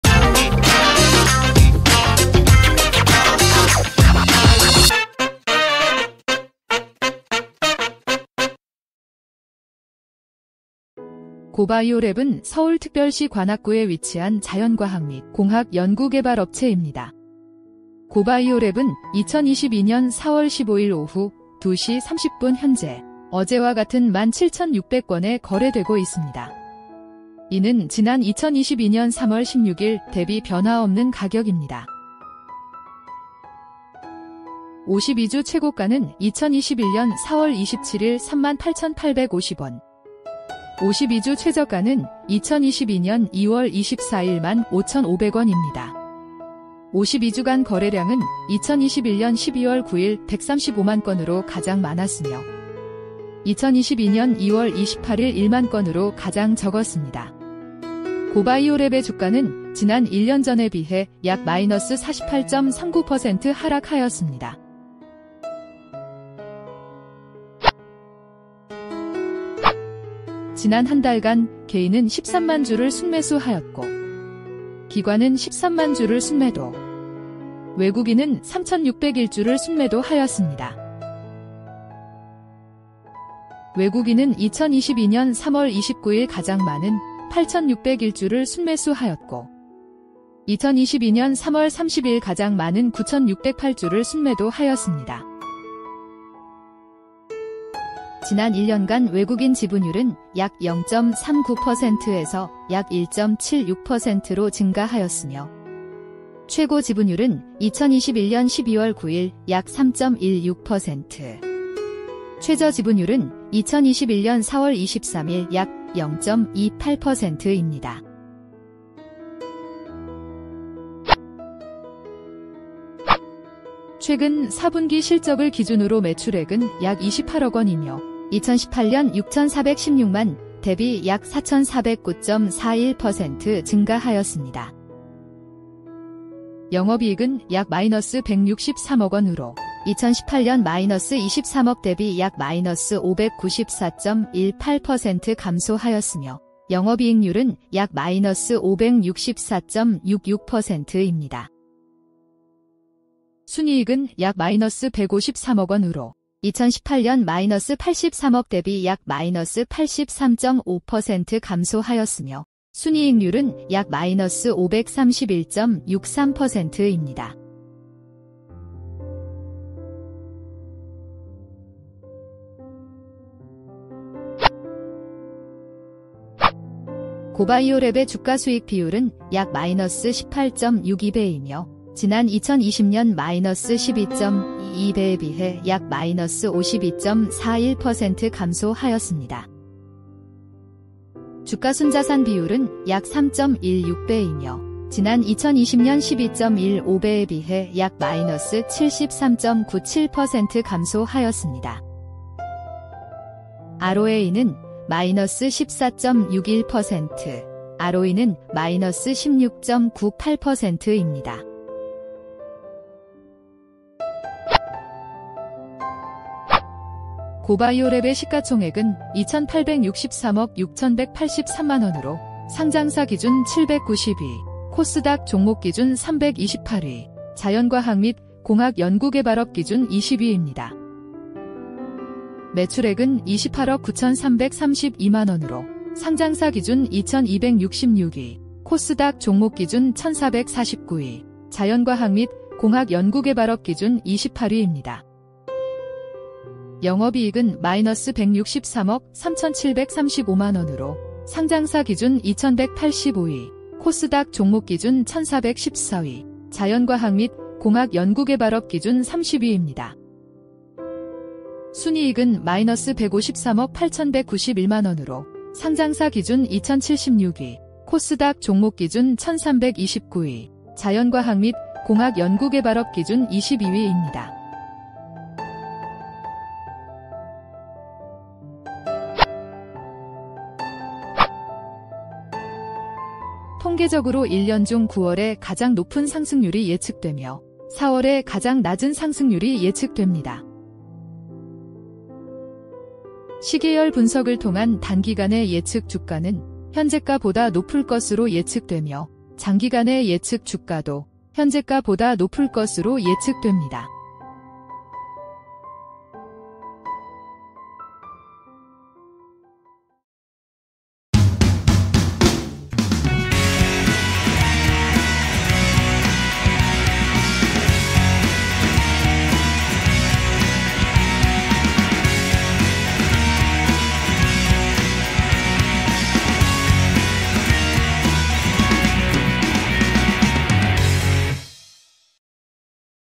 고바이오랩주가분석(348150) 2022년 04월 15일기준
고바이오랩은 서울특별시 관악구에 위치한
자연과학 및 공학 연구개발업업체 입니다.
고바이오랩은 2022년 04월 15일 오후 02시 30분 현재
어제와 같은 17,600원에 거래되고 있습니다.